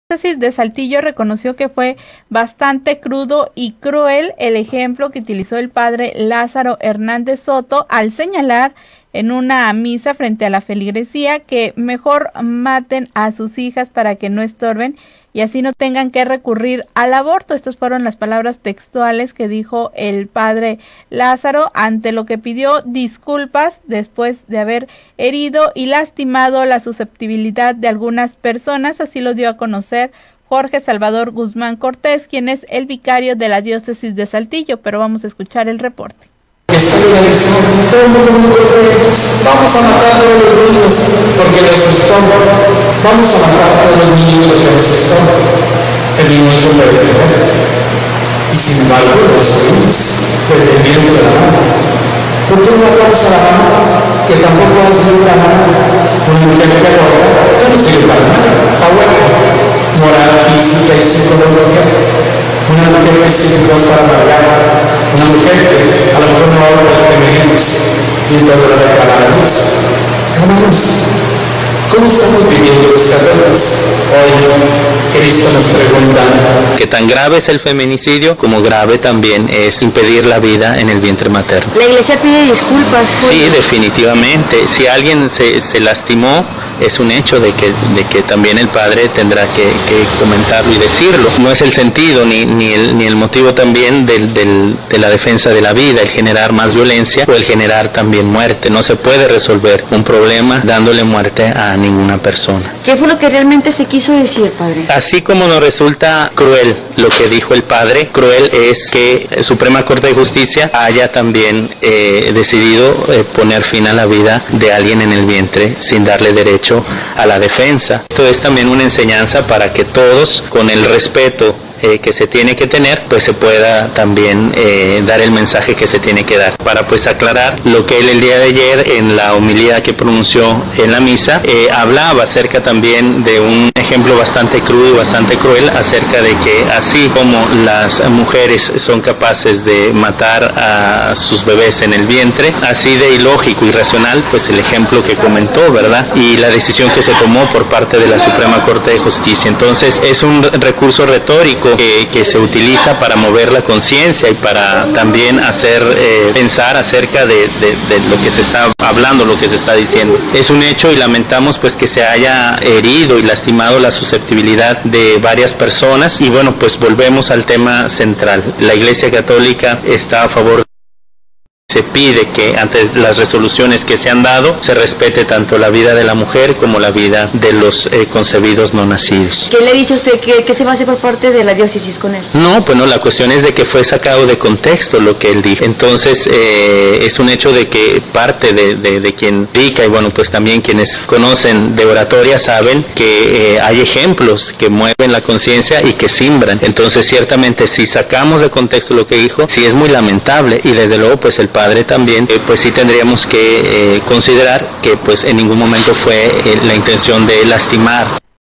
Monclova La Diócesis de Saltillo reconoció que fue bastante cruel el ejemplo que utilizó el padre.
Monclova La Diócesis de Saltillo reconoció que fue bastante crudo y cruel el ejemplo que utilizó el padre Lázaro Hernández Soto al señalar en una misa a la feligresía: “mejor maten a sus hijas para que no estorben y así no tengan que recurrir al aborto”, ante lo que pidió disculpas por haber herido y lastimado la susceptibilidad de algunas personas, dio a conocer Jorge Salvador Guzmán Cortés, vicario de la Diócesis de Saltillo.